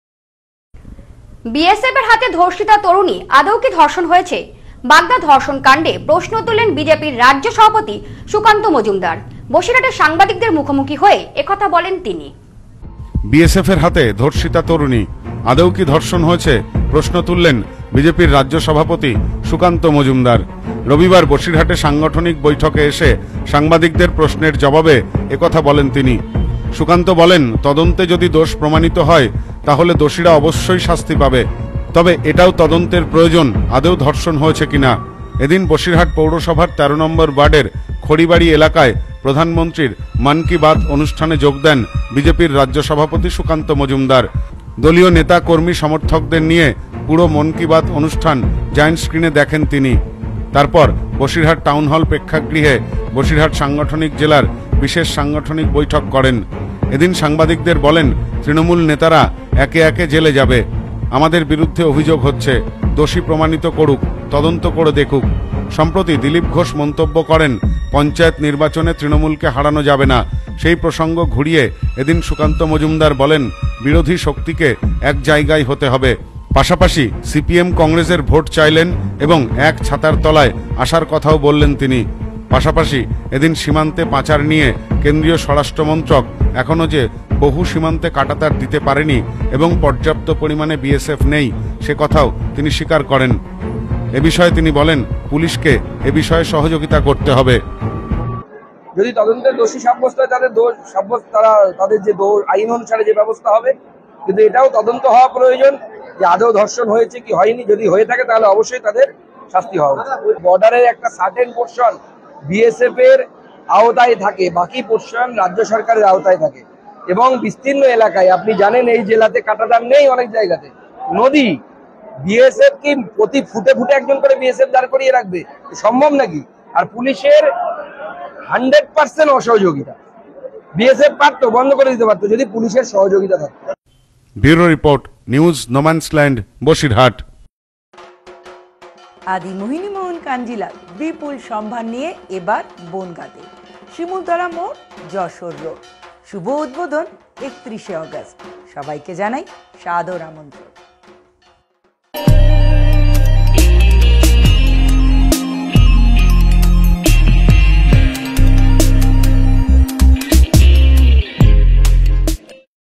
'তরুণী আদৌ কি ধর্ষণ হয়েছে?'বাগদা ধর্ষণকাণ্ডে প্রশ্ন তুললেন বিজেপির রাজ্য সভাপতি৷
'বিএসএফের হাতে ধর্ষিতা তরুণী আদৌ কি ধর্ষণ হয়েছে? বাগদা ধর্ষণকাণ্ডে প্রশ্ন তুললেন বিজেপির রাজ্য সভাপতি সুকান্ত মজুমদার। এদিন বসিরহাটের সাংবাদিকদের মুখোমুখি হয়ে এ কথা বলেন তিনি।